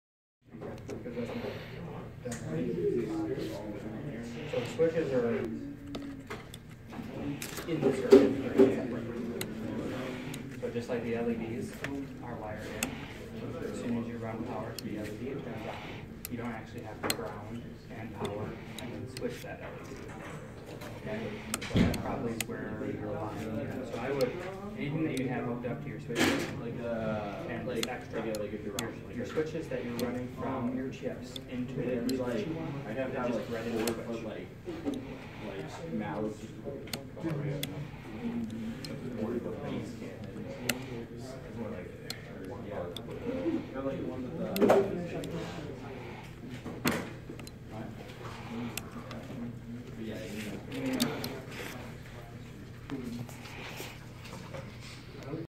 My Wired Up Bread Board, and It Works
Took me a while draw up the complex version based off of my truth table, and then it took me longer to draw up the simplified version which turned out that I had the second most simplified version. I used 8 gates, and 4 switches. And it all works.